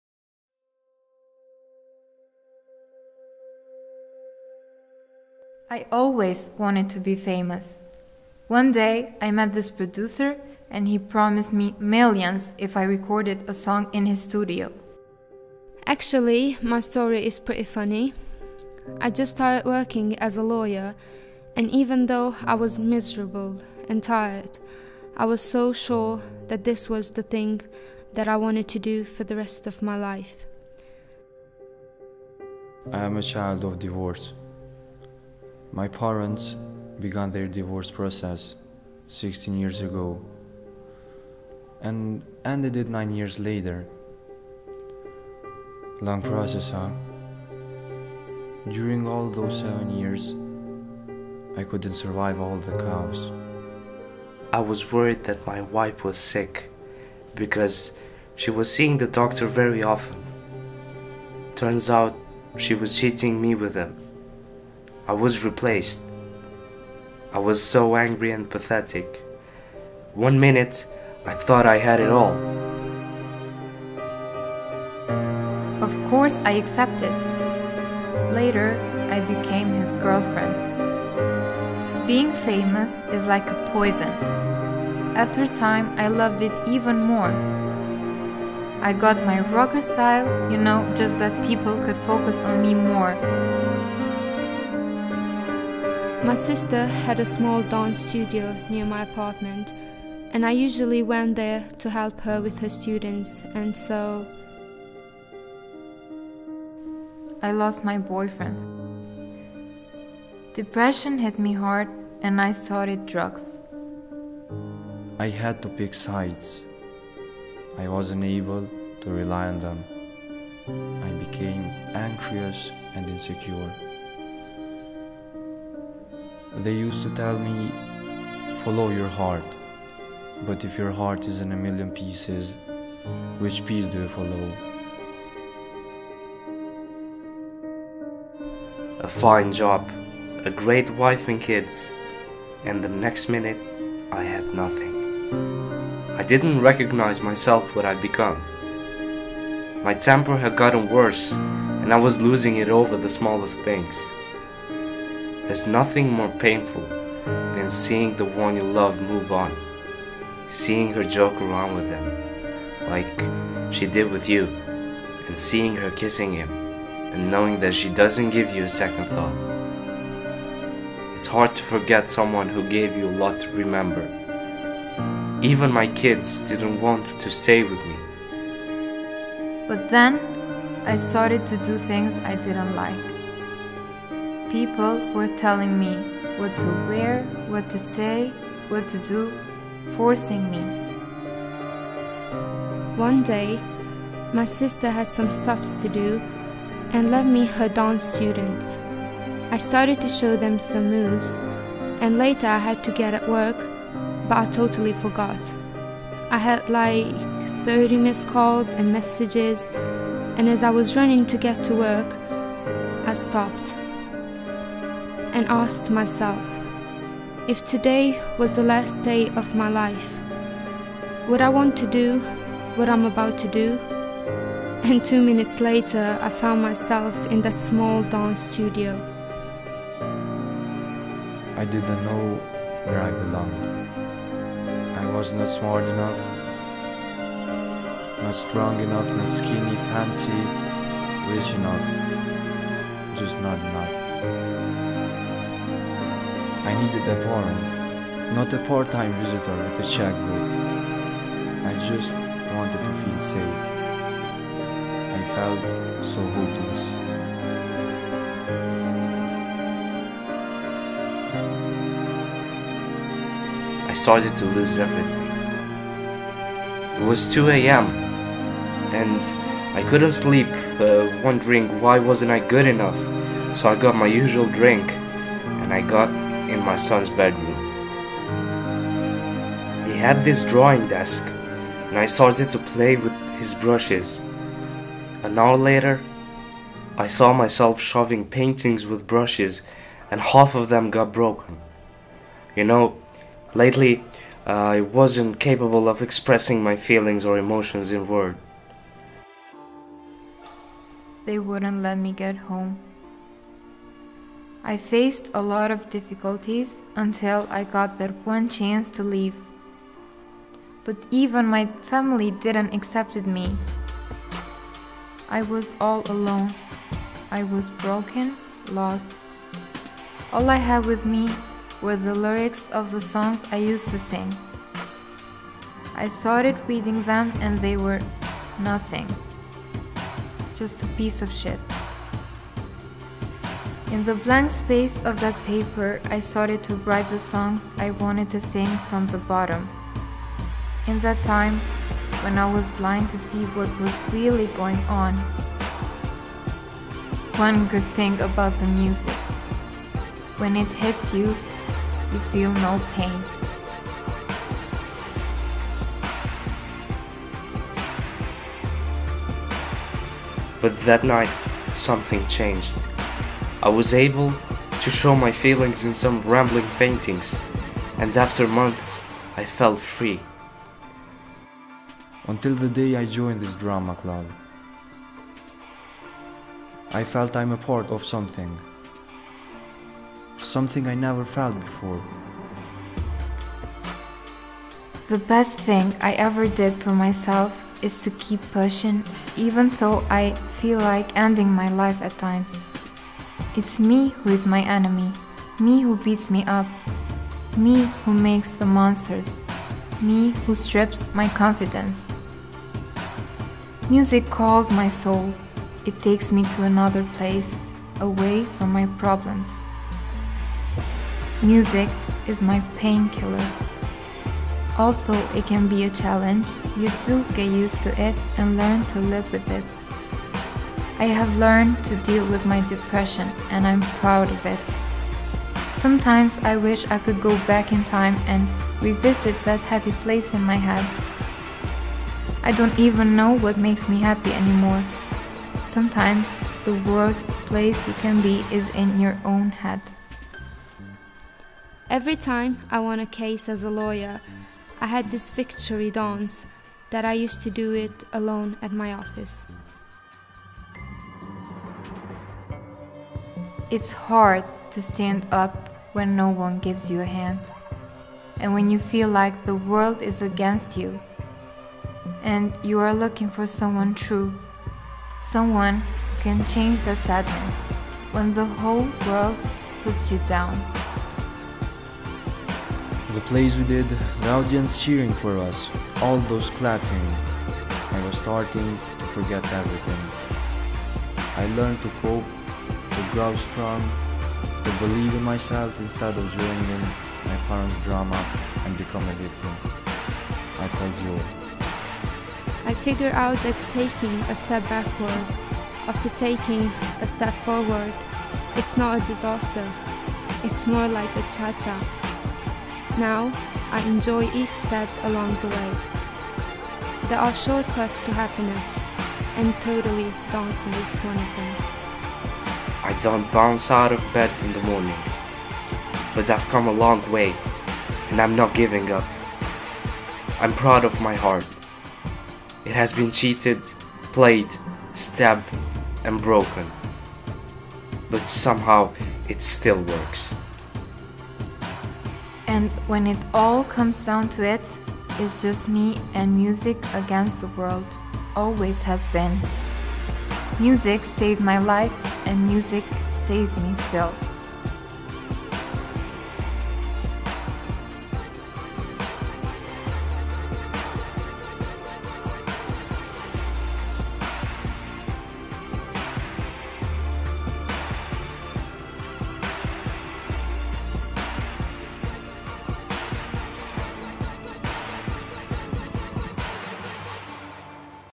A.R.T. Admire Rhythm Track
Ky film është krijuar nga studentët e vitit të parë të Departamentit Gjuhë Angleze në Kolegjin Universum. Filmi ka të bëjë me ndikimin e artit në jetët e personazheve. Për shpëtimin dhe fisnikrimin e shpirtit përmes muzikës, pikturës, aktrimit dhe kërcimit.

Regjia dhe skenari:
Era Baliu

Aktorët:
Arlinda Sejdiu
Bulza Dërmaku
Driol Hoxhaj
Eros Qerimi